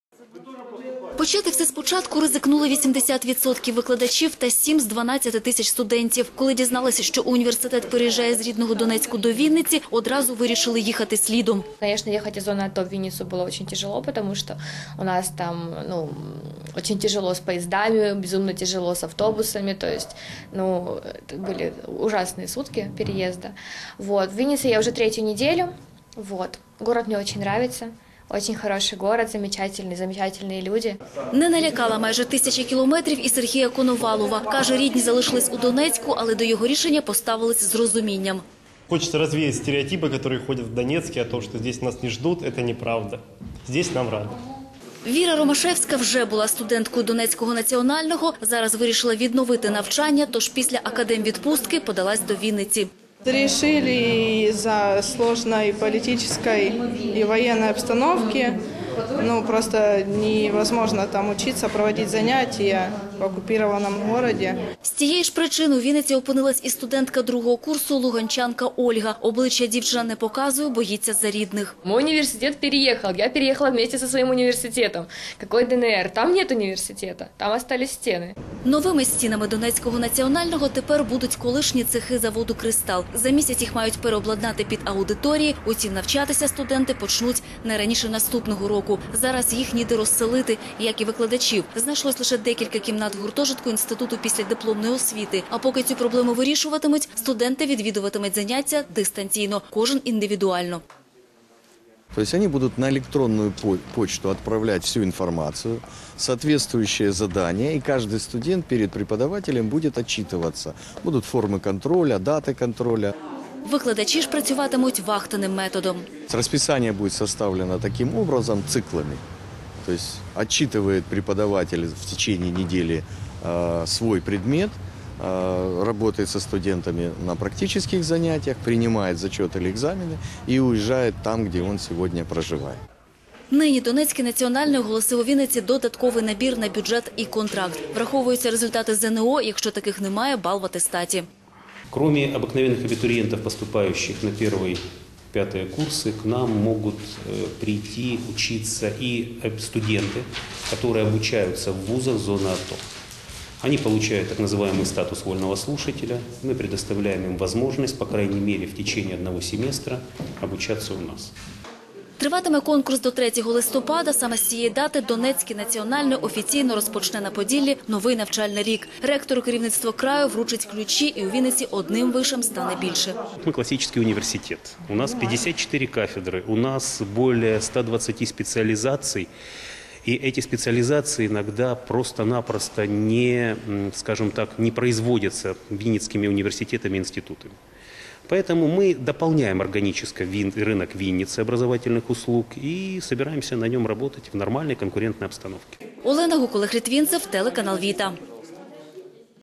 Телеканал ВІТА новини 2014-10-30 Наукові переселенці - пускають коріння у Вінниці
Донецький національний університет повним ходом готується у Вінниці до офіційного відкриття. Під аудиторії ВИШу міська влада віддала колишню будівлю заводу Кристалл. Поки виробничі приміщення реконструюють, донецький універ добирає студентів.